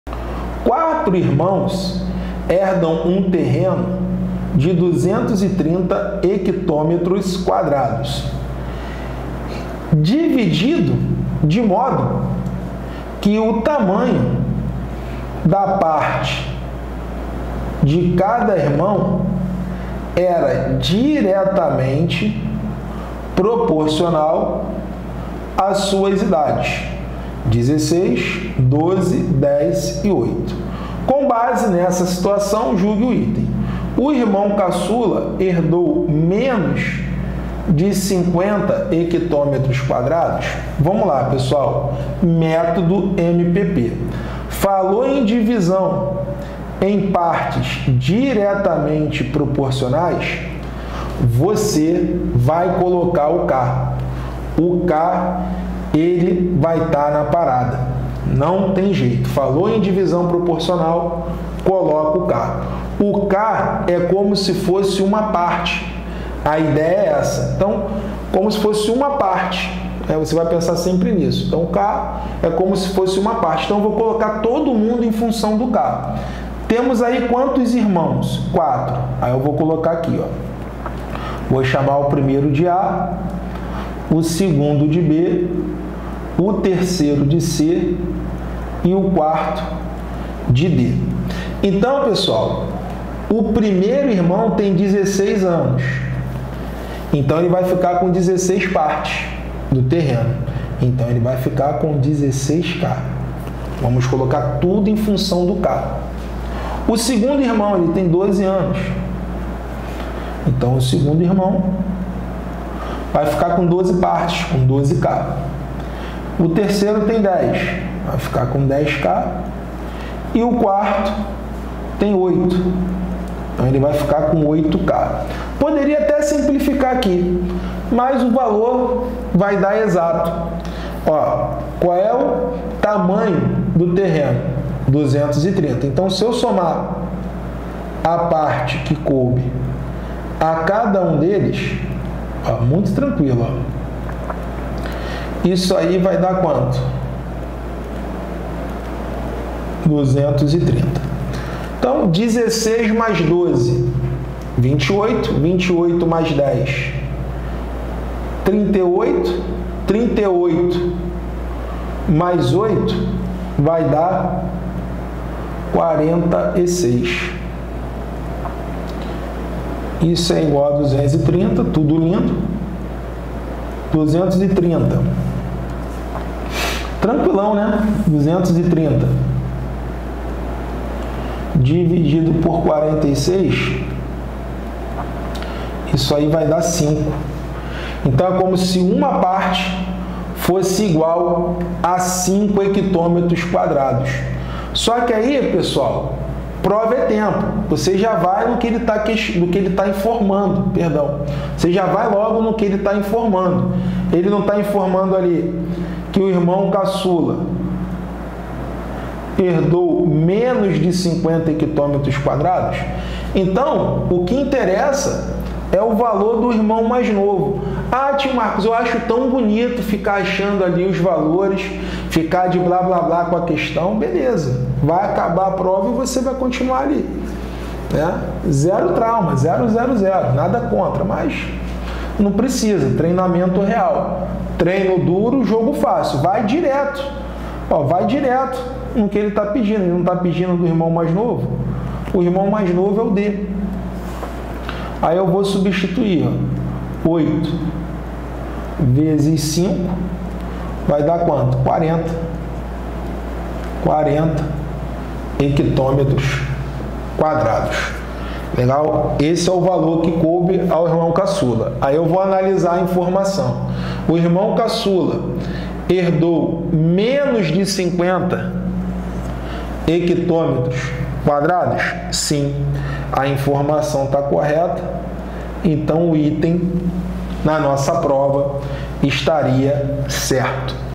Matemática para PM CE |Dica 37| Veja Como Resolver Divisão Proporcional
Você não sabe Divisão Proporcional para PM CE ?

Então você precisa aprender com esse vídeo onde nosso professor, Marcão, mostra Divisão Proporcional para PM CE.

Você terá um passo a passo para aprender Divisão Proporcional para PM CE e nunca mais errar nenhuma questão.

Agora basta dar o play no vídeo e aprender com o Método MPP Divisão Proporcional para PM CE.